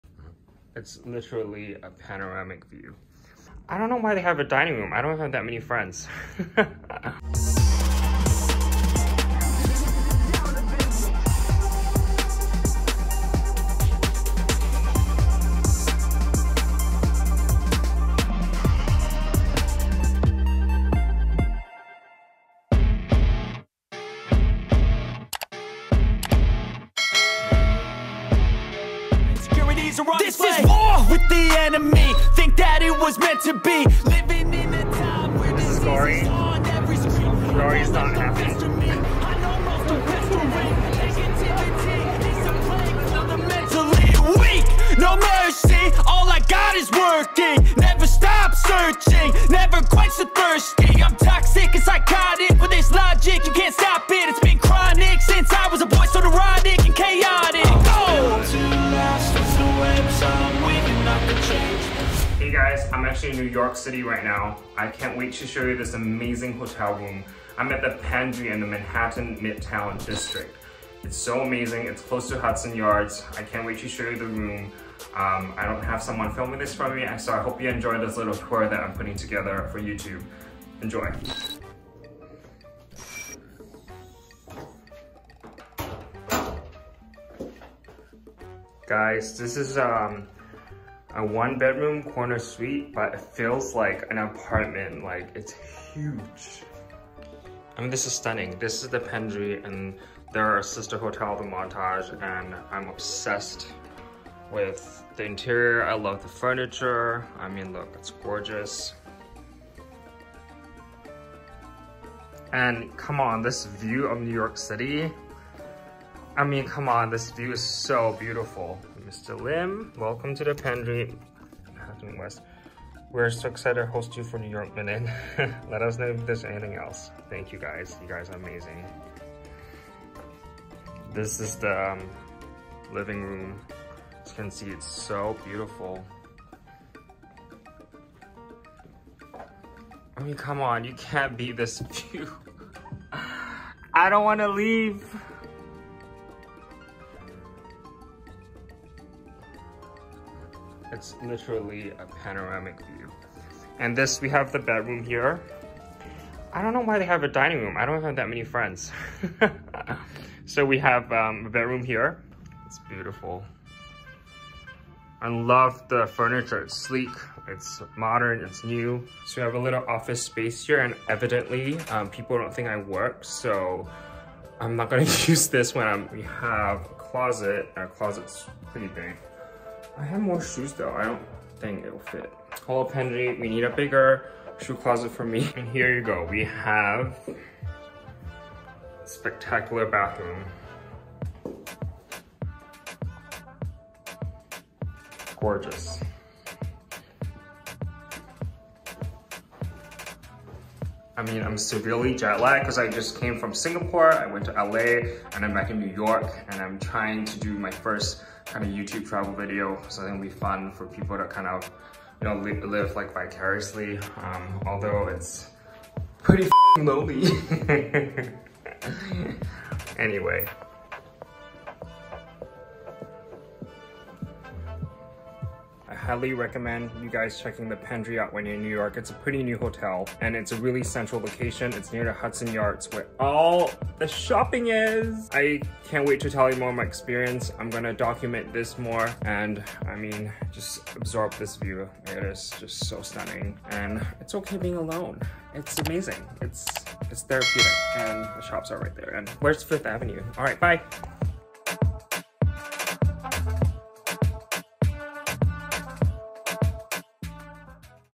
NYC Hotel Tour | KANE LIM
From Singapore to LA and now to New York! What better way to start off my YouTube journey than with a private jet tour and a hotel room tour at the Pendry Manhattan West?

Welcome to Kane’s world, where you get to see a glimpse of my life with and without the bling.